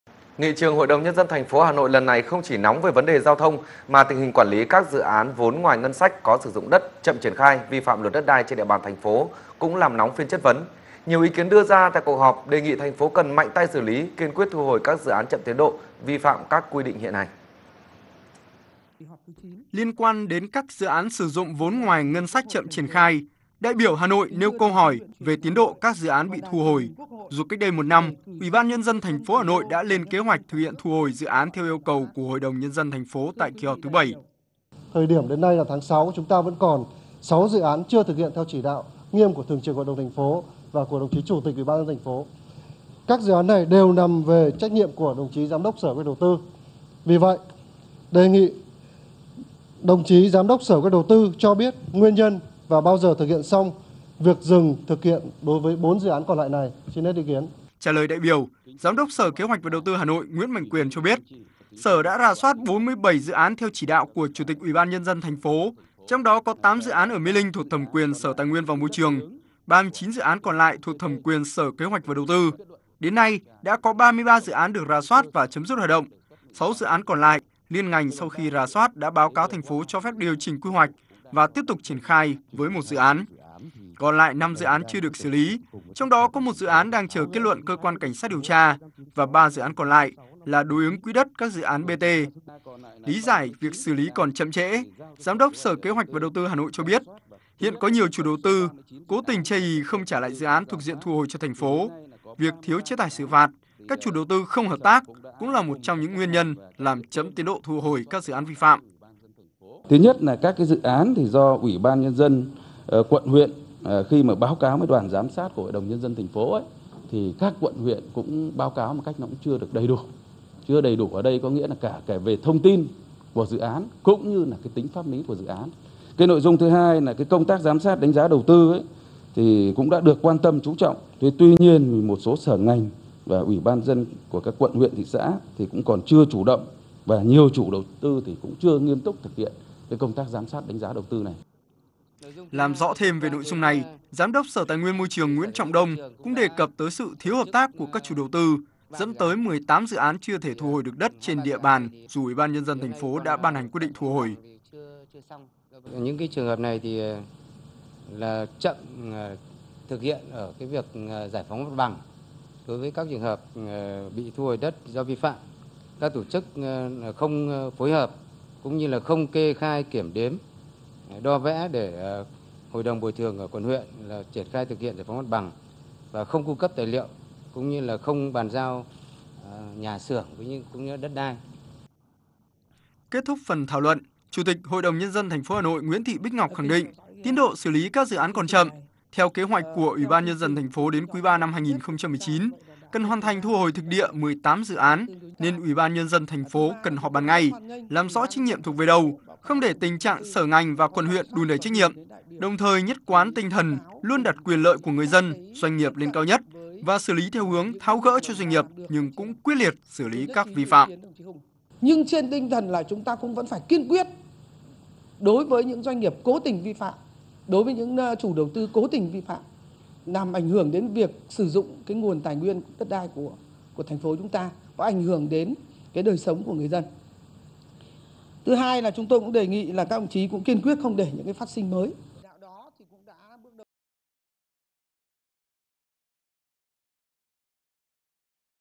Kiên quyết xử lí doanh nghiệp cố tình "treo" dự án
VTC1 | Giám đốc Sở Kế hoạch&Đầu tư Hà Nội khẳng định đã rà soát và sẽ kiên quyết đối với các dự án chậm tiến độ, dự án treo.